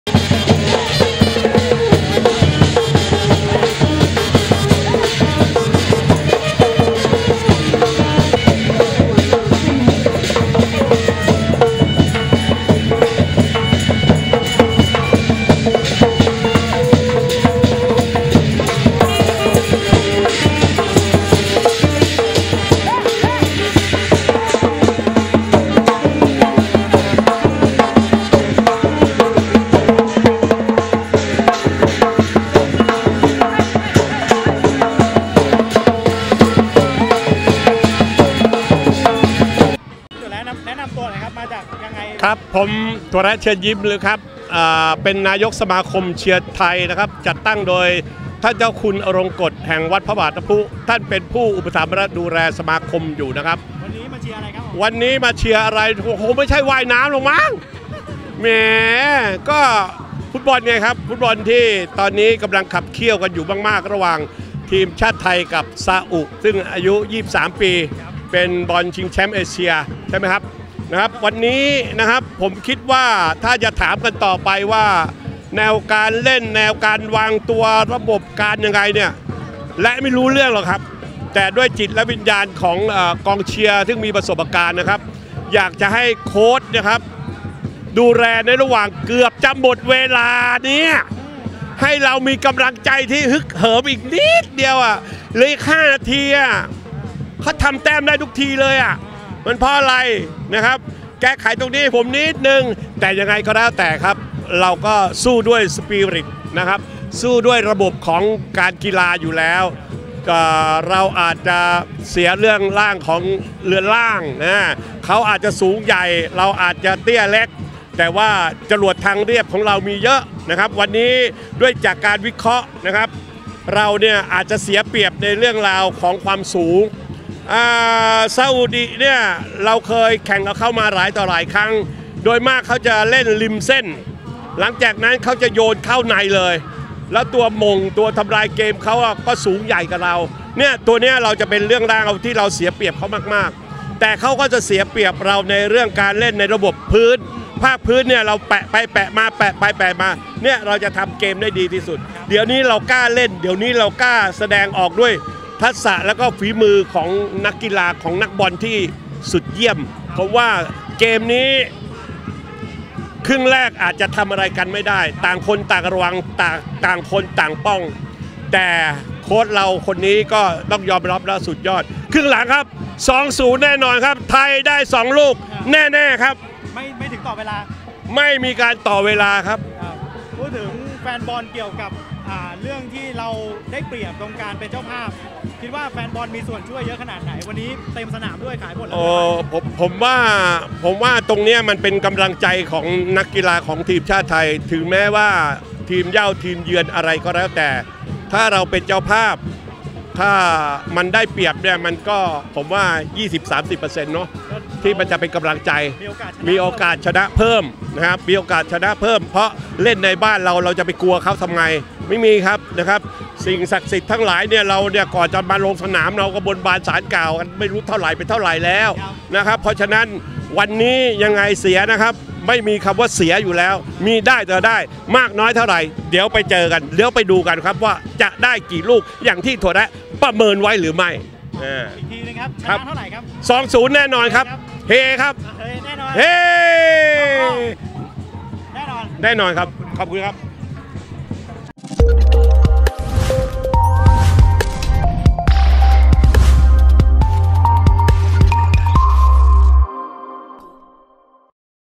กองเชียร์ทัพช้างศึก พร้อมส่งกำลังใจสู้ศึกนัดสำคัญ
ติดตามข่าวสารความเคลื่อนไหวกีฬาในประเทศและนอกประเทศ